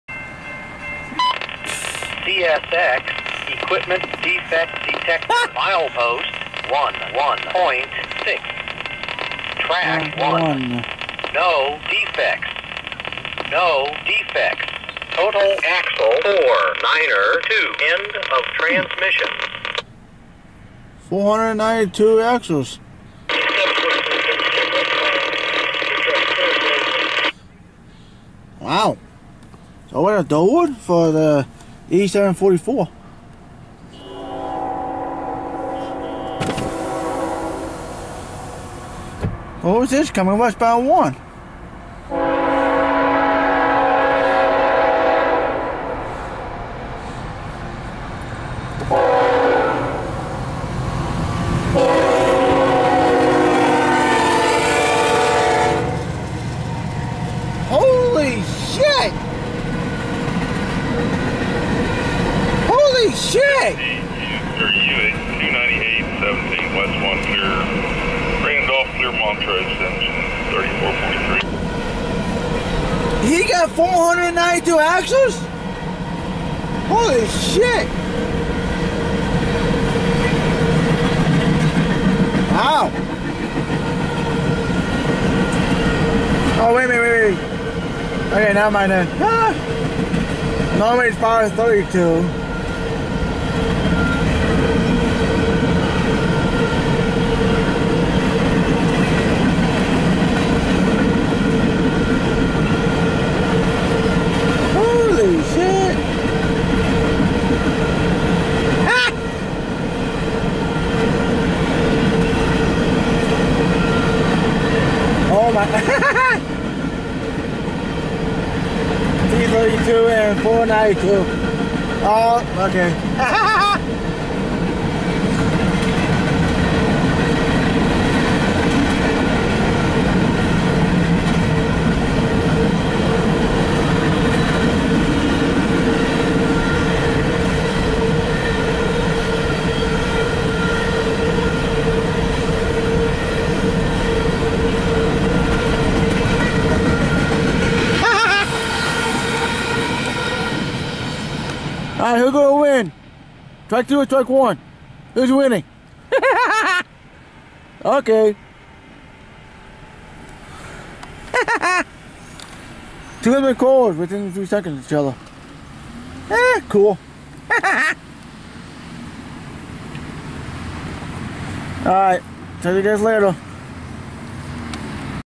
#2365 CSX T298 Empty Coal train
CSX T298 Empty Coal train is heading westbound on track 1. you can tell i am getting a little tired.